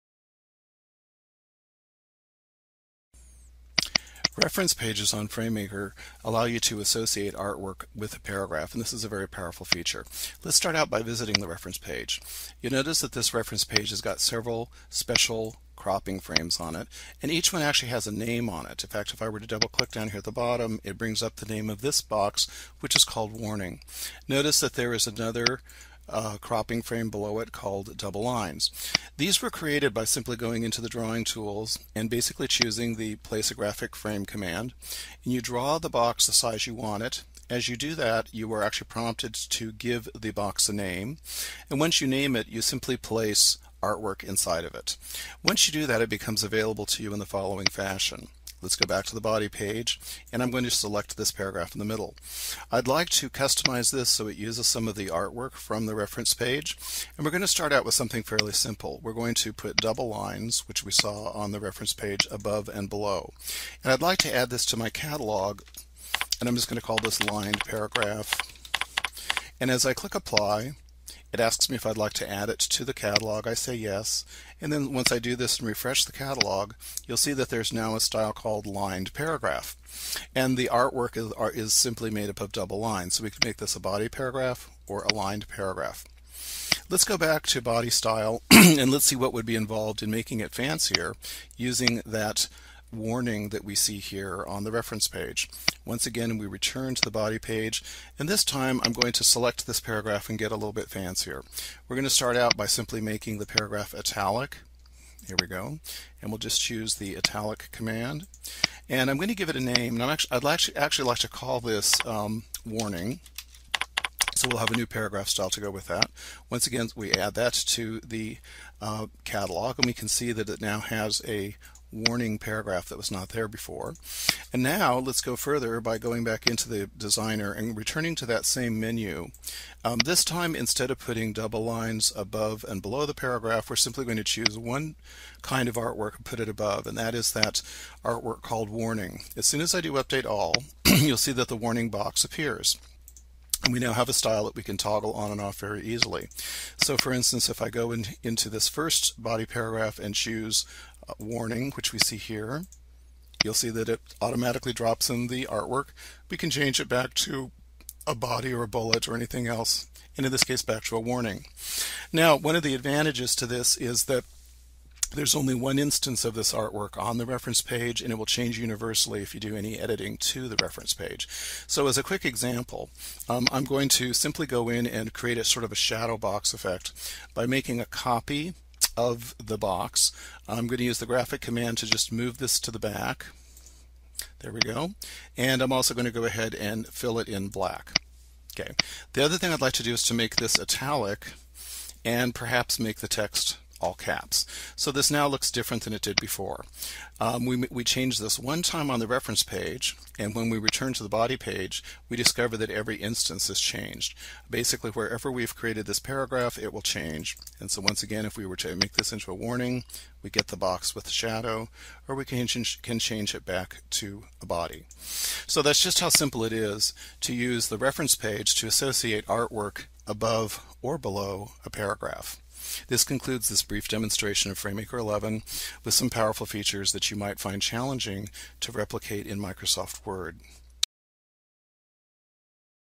FrameMaker and Word: Reference Page Art for Automatic Warnings
FrameMaker 11's Reference Pages allow you to associate text and artwork with paragraph formatting. For instance, you can place horizontal lines above or below a paragraph, or have a "Caution" box automatically placed above a paragraph. Writers cannot edit this artwork on the body page; one must visit the Reference Page in order to make a change. If the user revises the artwork or text in a special frame on the Reference Page, all instances associated with paragraph styles will universally update throughout the document.